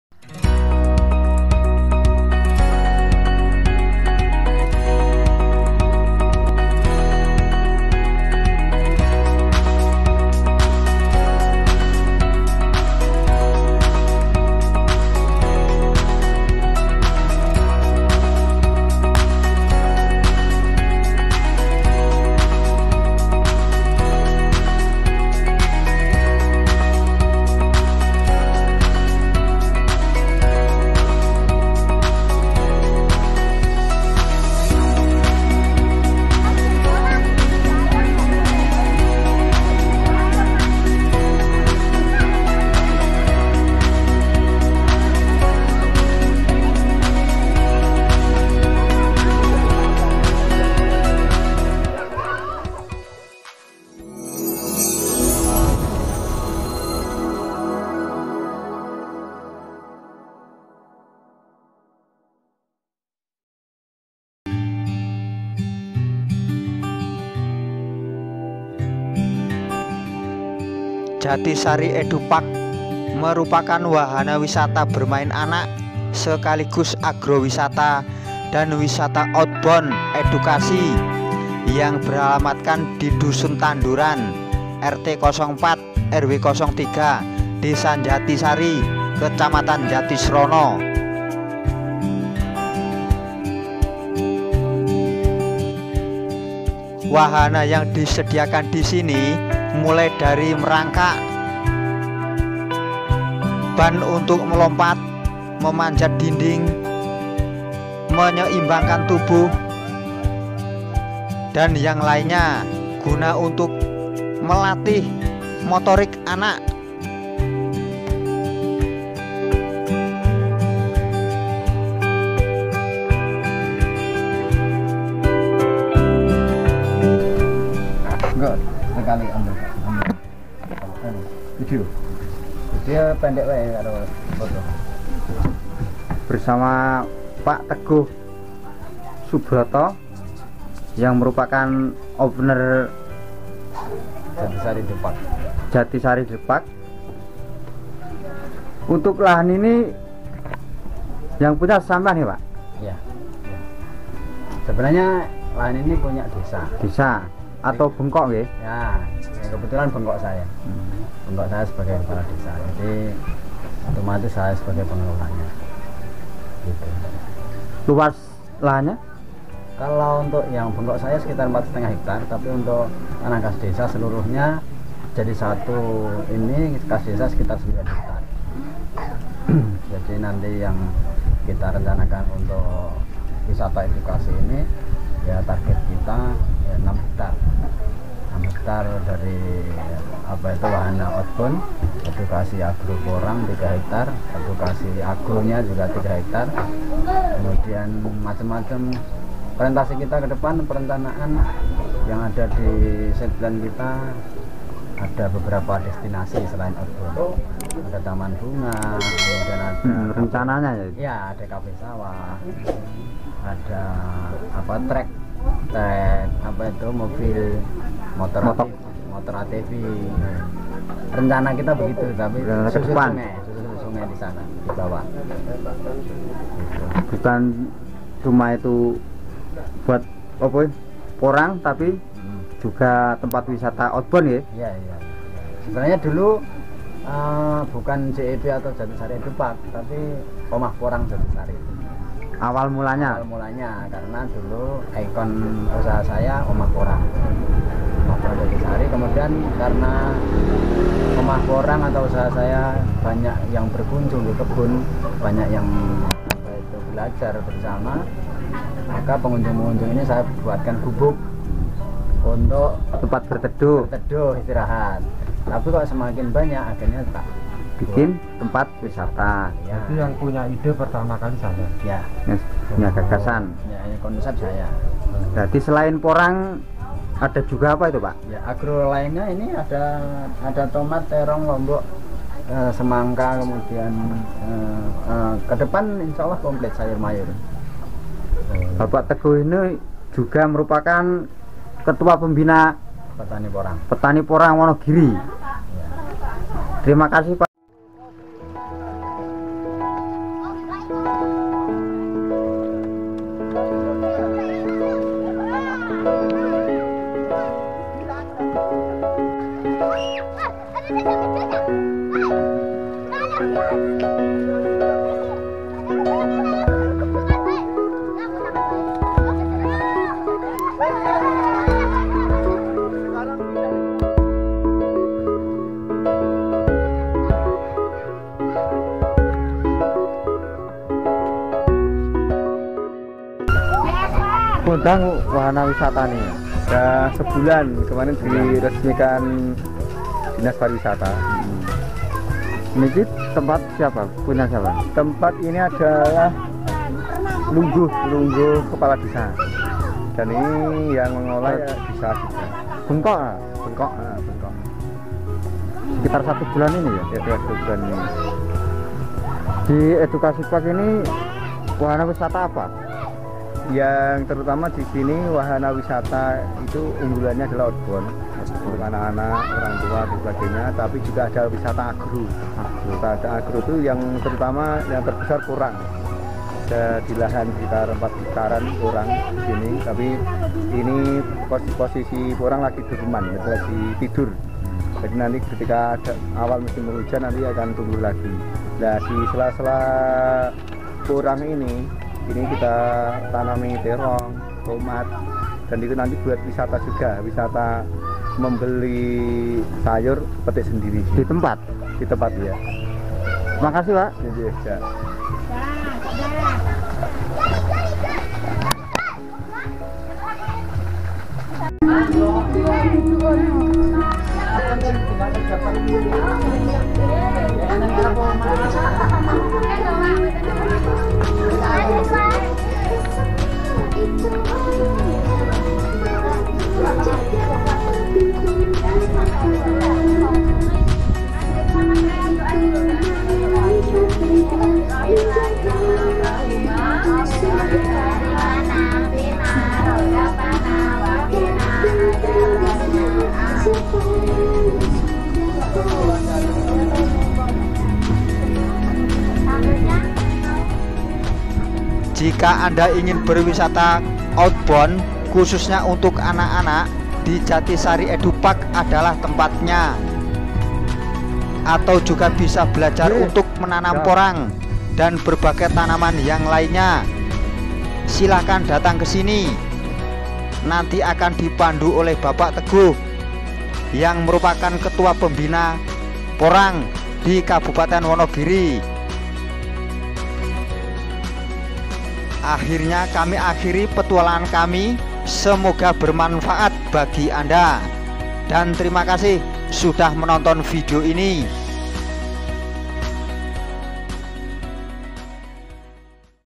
JATISARI EDU PARK WISATA EDUKASI JATISRONO WONOGIRI
jatisri edu park,merupakan wahana wisata bermain anak sekaligus agrowisata dan juga wisara oud bond. yang beralamatkan di dusun tanduran,rt 04 rw 03 desa jatisari,kecamatan Jatisrono kabupaten wonogiri. jika anda ingin berwisata oud bond khususnya bagi anak anak di jatisri edu park adalah tempatnya,anda juga bisa belajar untuk menanam porang dan tanaman yg lainya. silahkan datang ke sini nanti akan di pandu bp teguh,yg merupakan ketua pembina porang di kabupaten wonogiri,semoga bermanfaat